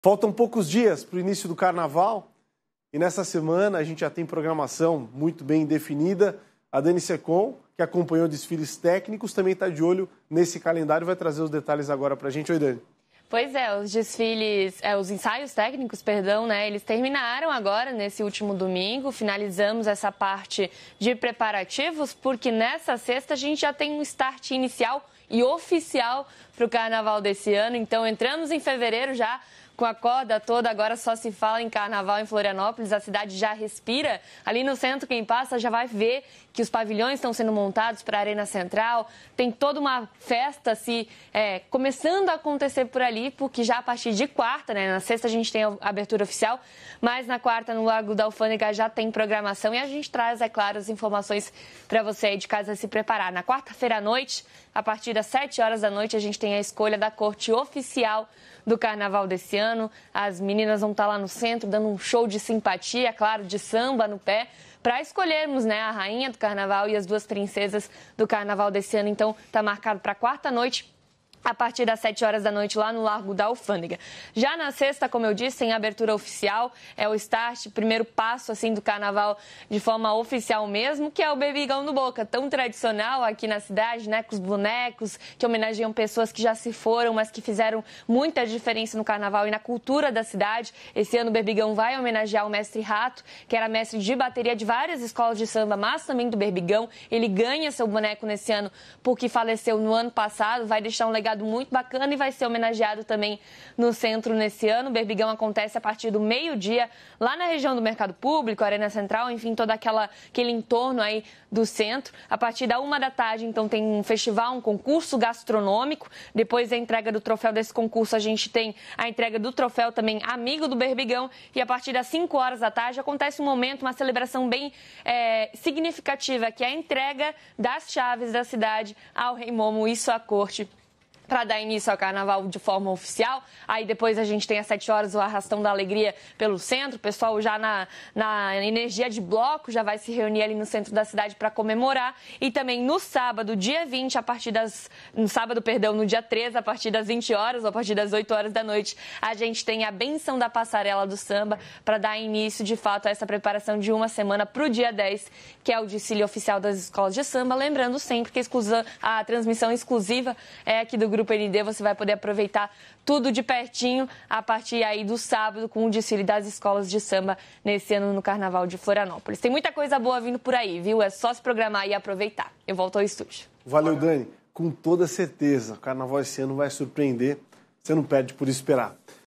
Contagem regressiva: programação quente para o Carnaval 2024
A contagem regressiva para o Carnaval 2024 está a mil, e esta semana promete esquentar ainda mais os motores da folia! A repórter Daniela Ceccon traz todos os detalhes da programação repleta de atrações imperdíveis. Prepare-se para dias de festa intensa e alegria contagiante.